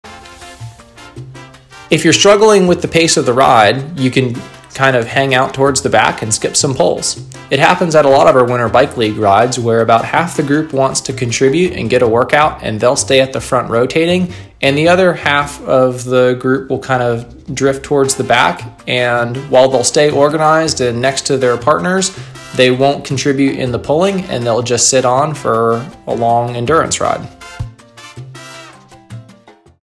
Asheville Winter Bike League Training Series: Dangling off the Back
About half of our riders want a full work out, so they stay towards the front of the pack and take turns "pulling." The other half wants to stay towards the back and enjoy more of an endurance ride. If you feel yourself struggling with the pace, stay organized but feel free to drift towards the back.